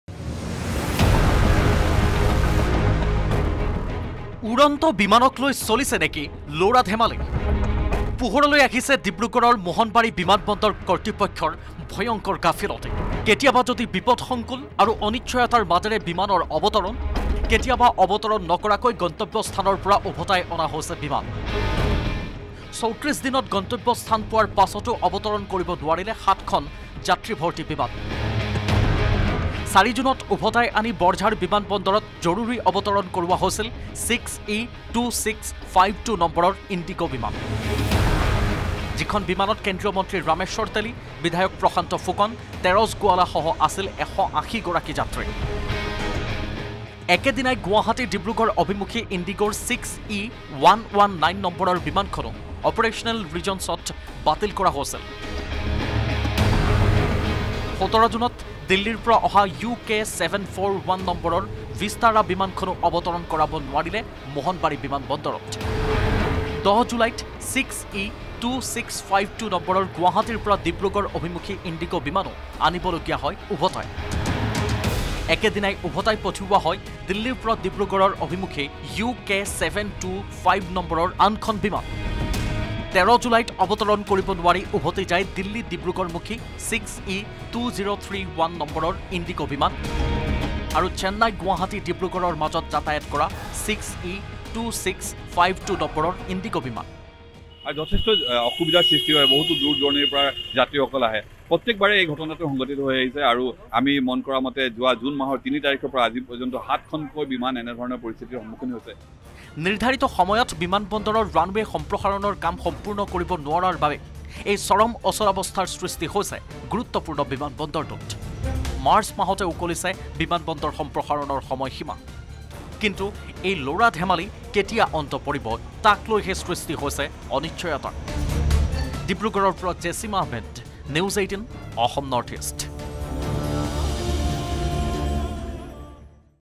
Dibrugarh Airport News : বিমানযাত্ৰীৰ জীৱনক লৈ কোনে খেলিছে হেতালি ?
Dibrugarh Airport News | বিমানযাত্ৰীৰ জীৱনক লৈ কোনে খেলিছে হেতালি? বিতৰ্কত ডিব্ৰুগড়ৰ মোহনবাৰী বিমানবন্দৰ কৰ্তৃপক্ষ। ৩৪ দিনত বিমানবন্দৰটোৰ পৰা অৱতৰণ কৰাব নোৱাৰি ওভতাই অনা হৈছে ৭খন বিমান